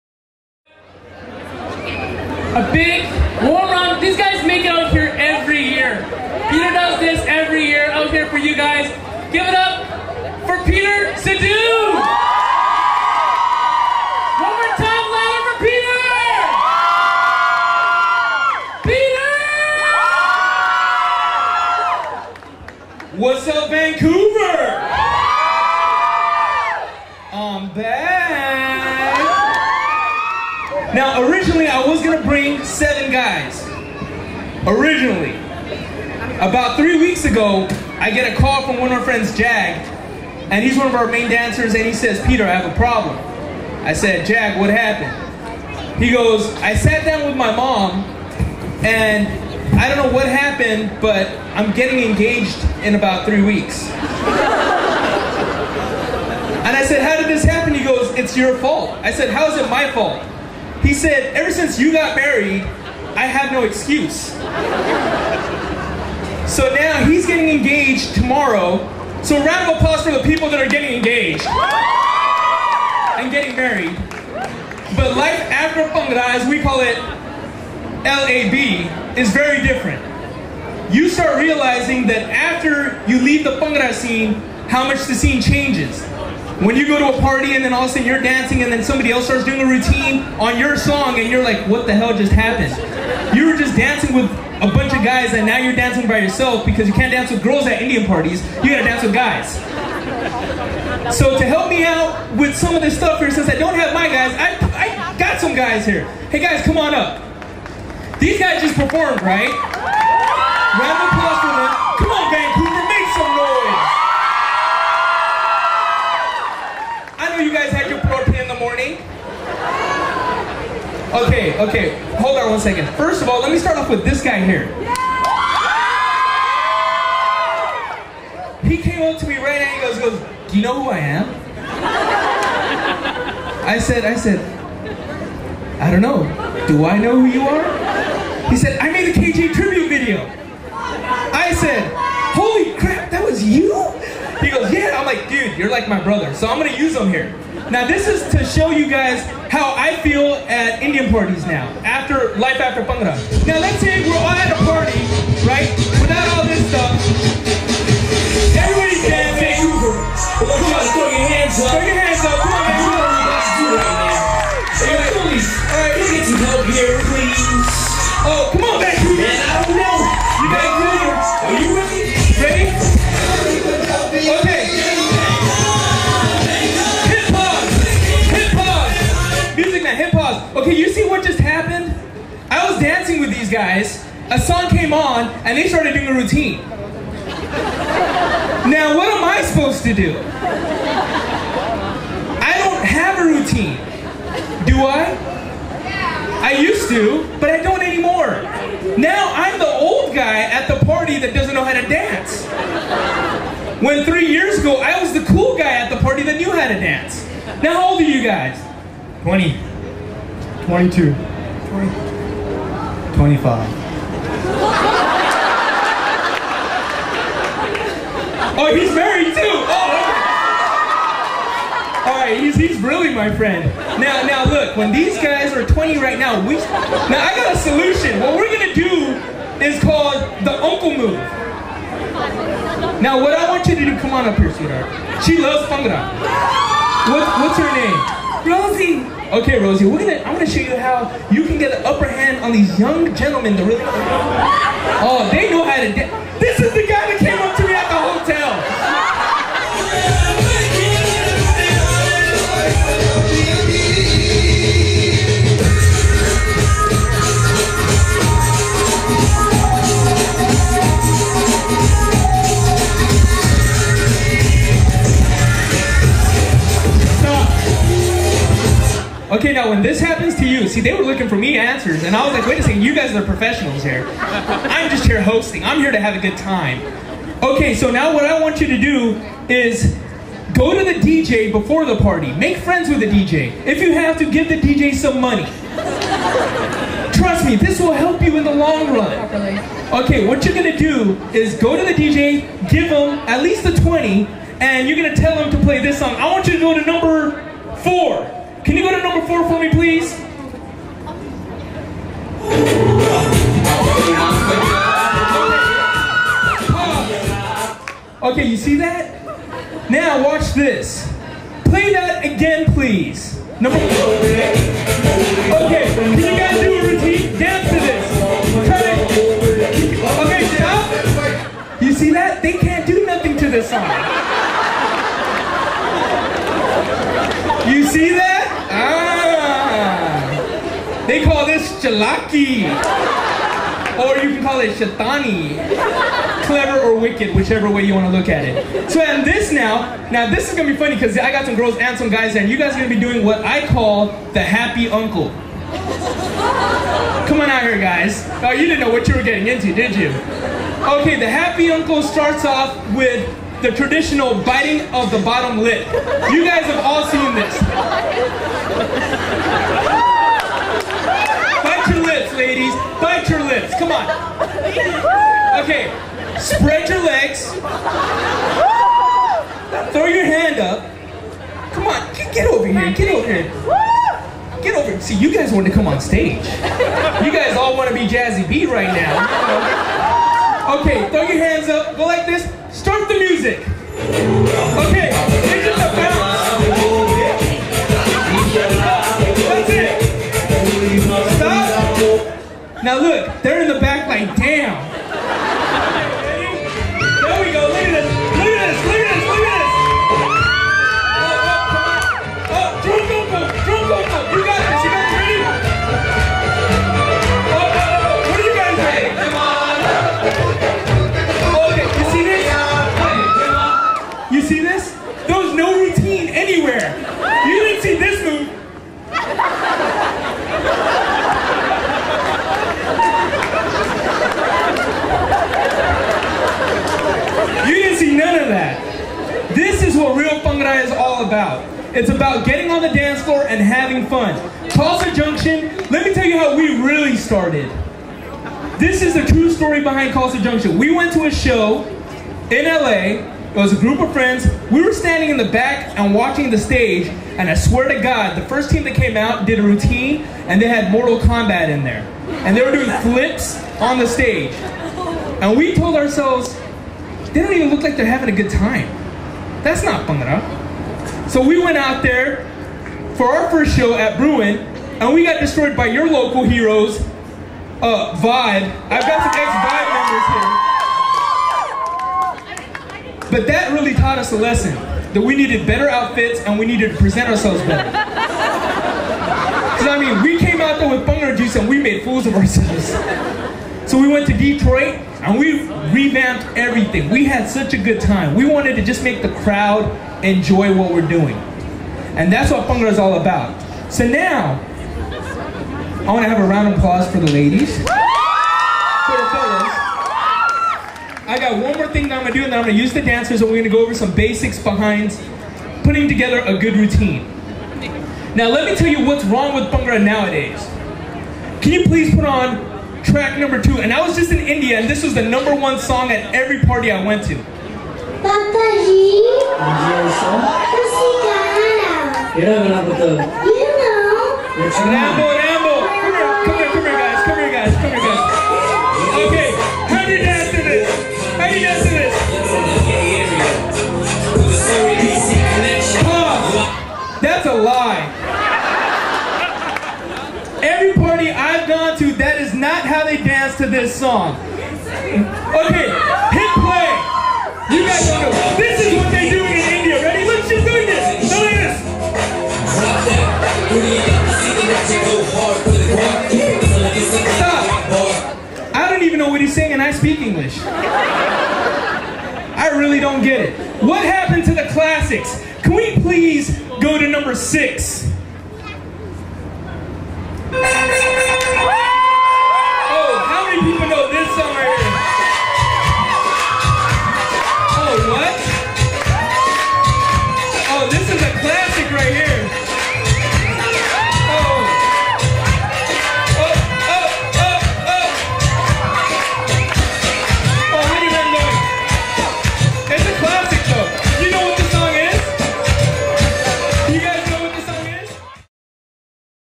Peter Sidhu @ VIBC Downtown Bhangra 2009 pt 1
Peter Sidhu of Khalsa Junction Bhangra holding a Bhangra Workshop at VIBC 2009 City of Bhangra Festival: Downtown Bhangra. Vancouver Art Gallery Plaza, Vancouver, BC, Canada. Feb 20, 2009.

Peter Sidhu teaching Bhangra moves, such as the "Happy Uncle," that would help you beat the pros at an Indian party. (An audience participation segment of Downtown Bhangra. Special guests:  Bhangra Republik (Calgary) team members.

Posted with permission from VIBC.